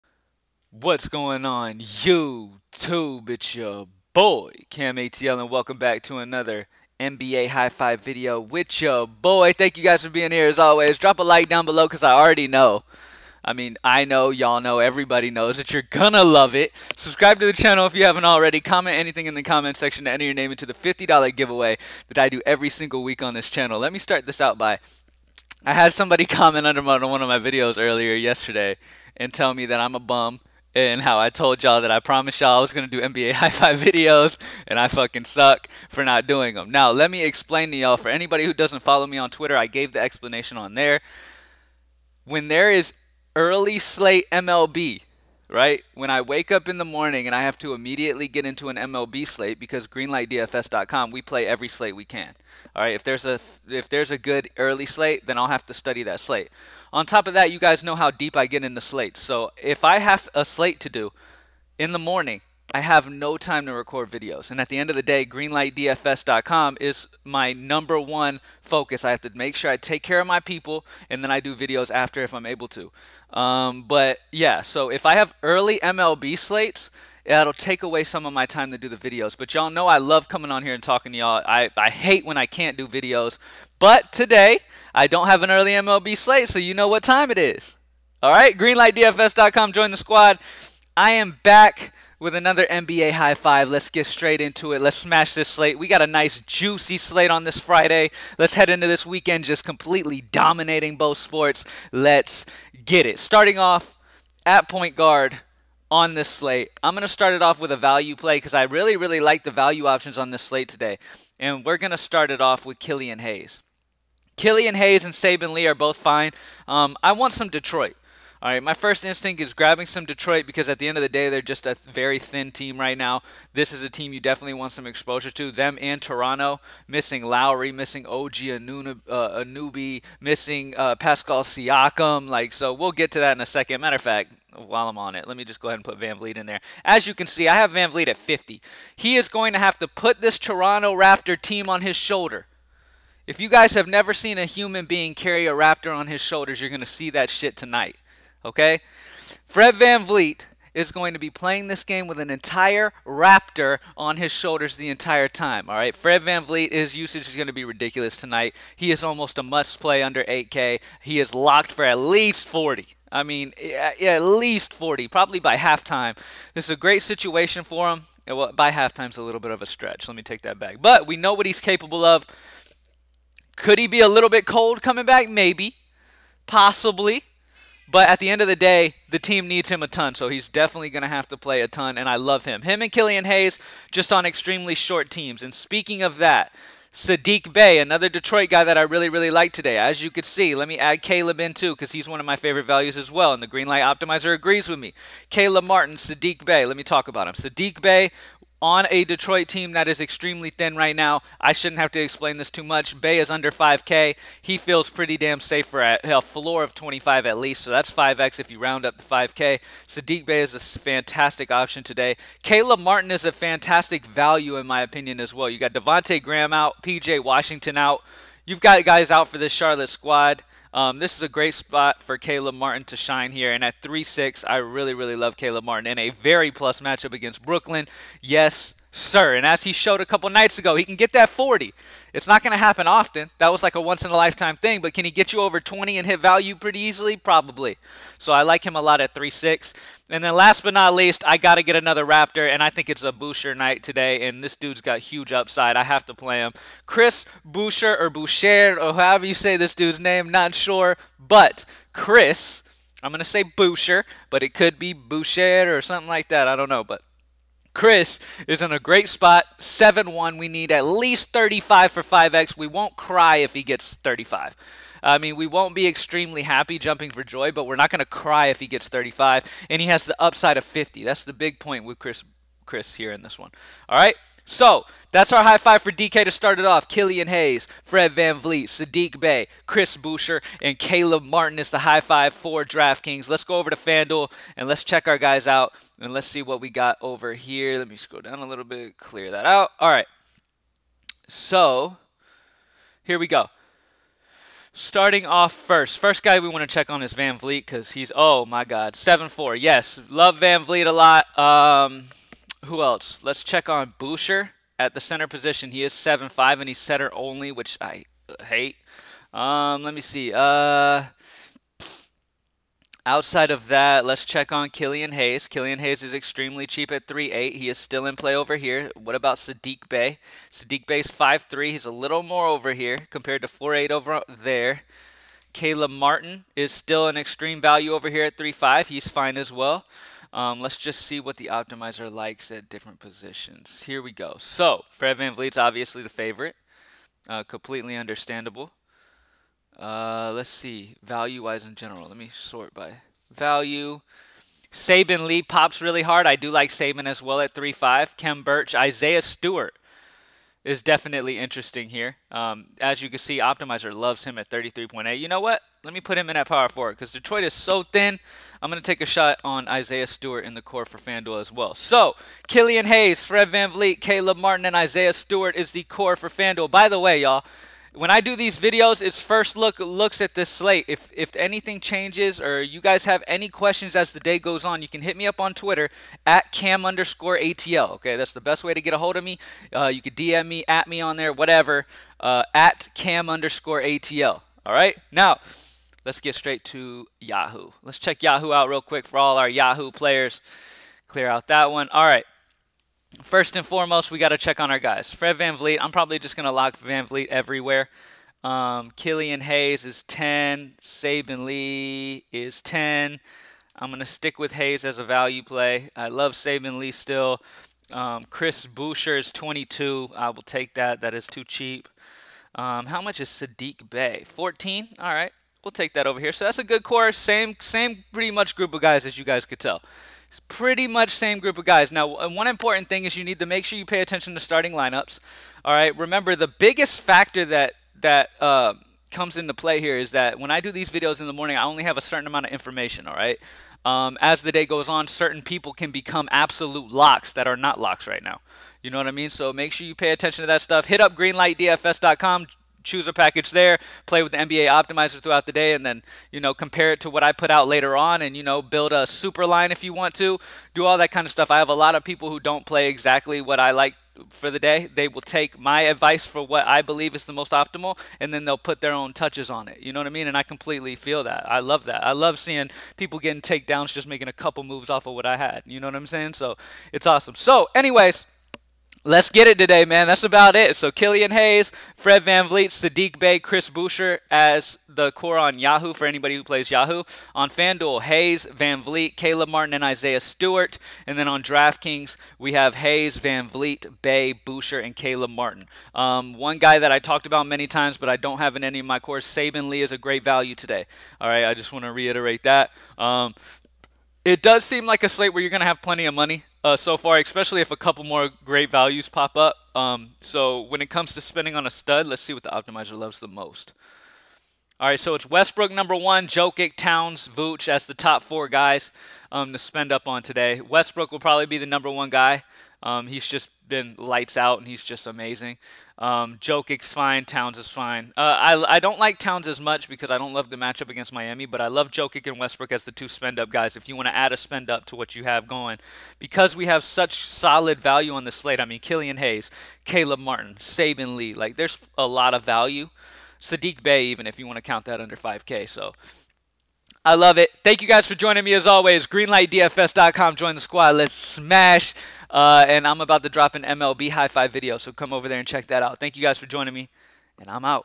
4/16/21 NBA DRAFTKINGS PICKS / HIGH FIVE
NBA IS BACK BABY! In this video i cover my fav core players for this 4/16/21 nba draftkings slate!!
It's going to be an exciting year!!